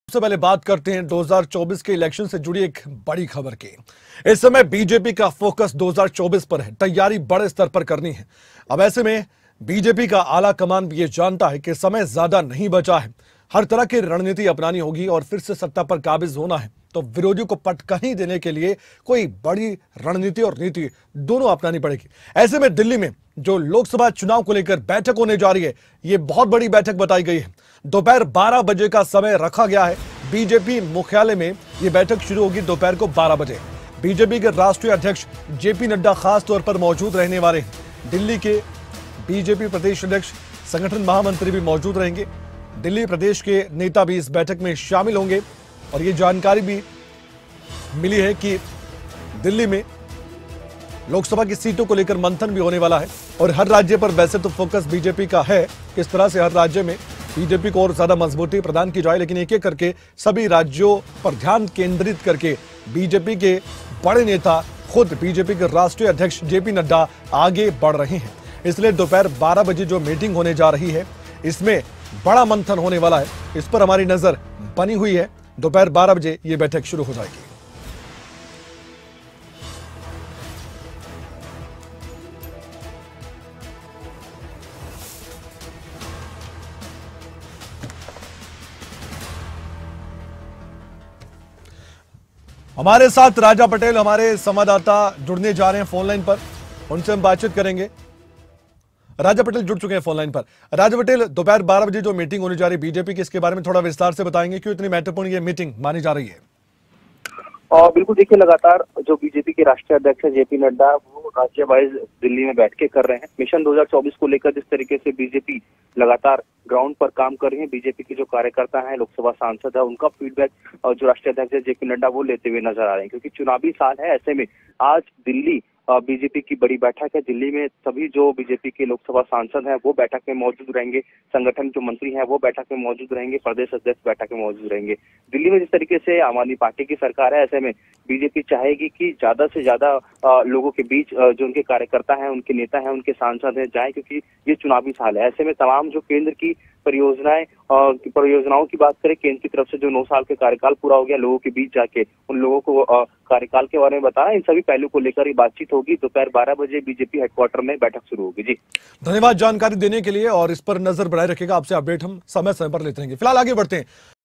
Mission-2024 पर BJP का फोकस, J.P. Nadda की अध्यक्षता में दिल्ली में अहम बैठक आज
मिशन-2024 पर बीजेपी का फोकस, जेपी नड्डा की अध्यक्षता में दिल्ली में अहम बैठक आज